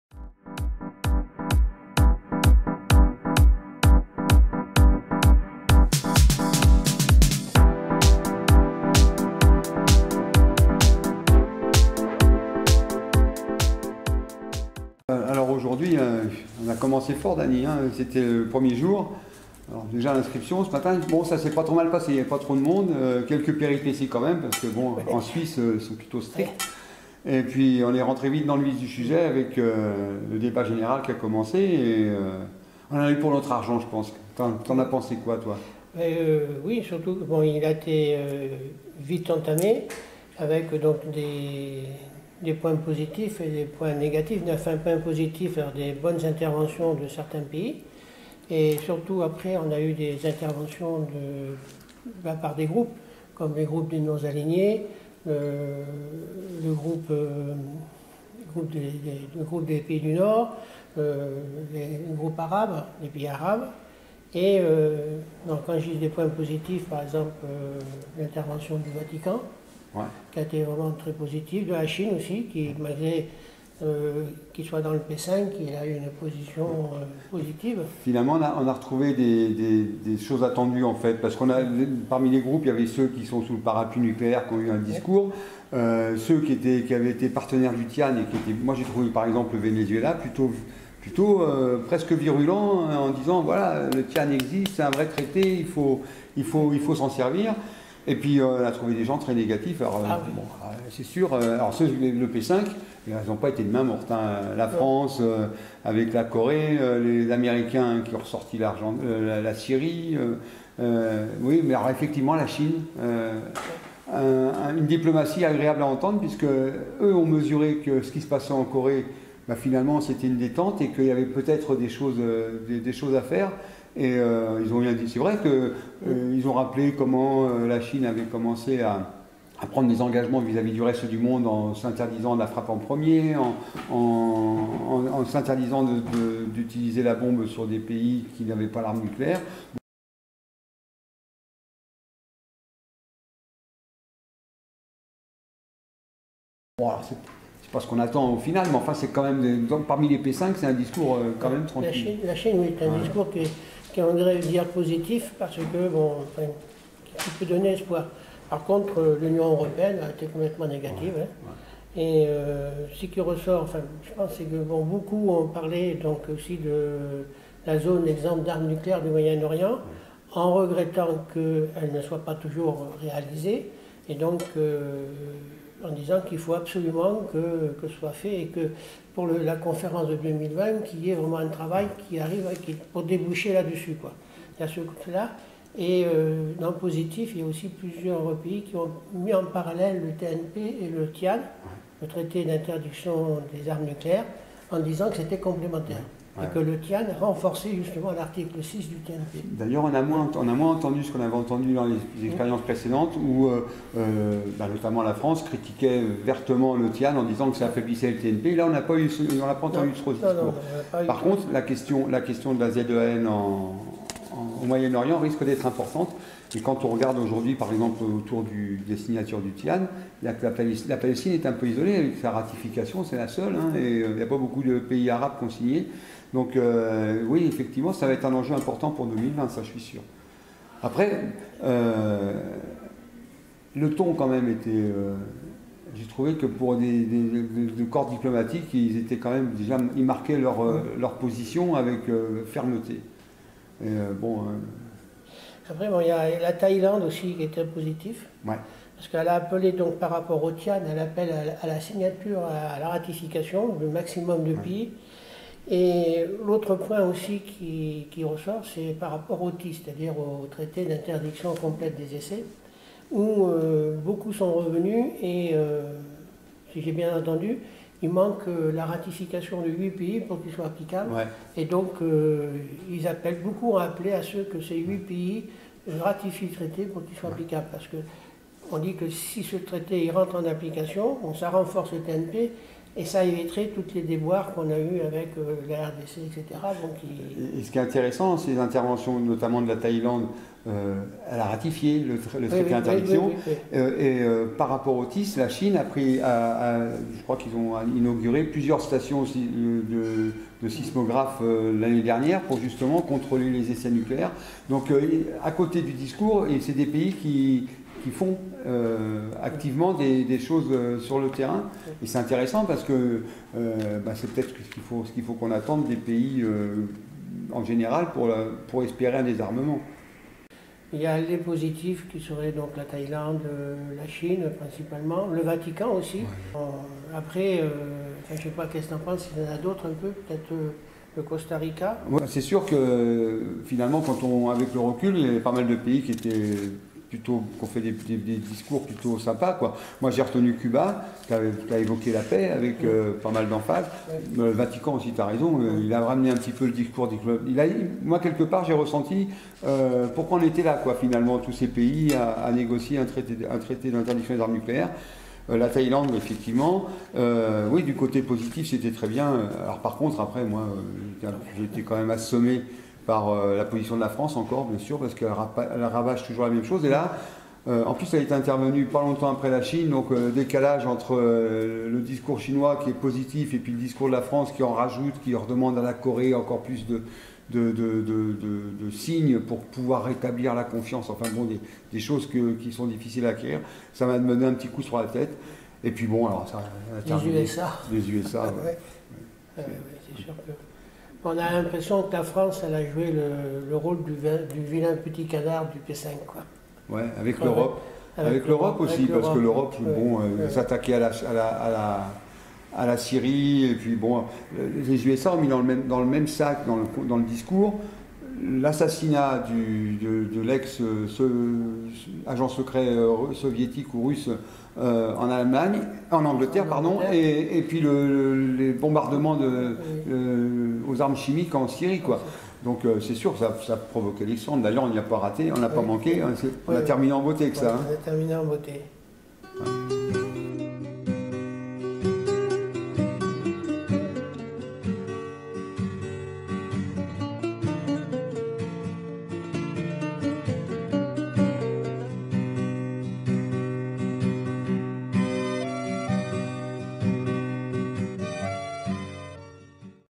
Genève 2018 : Flash info n°8
Délégation du Mouvement de la Paix à l'ONU, à Genève pour  la réunion de préparation à la Conférence d'examen du Traité sur la Non-Prolifération nucléaire.
Rendez-vous de la semaine : Flash Info N°8
L'ambiance de la première journée. Michel Dolot et Dany Allaire du Mouvement de la Paix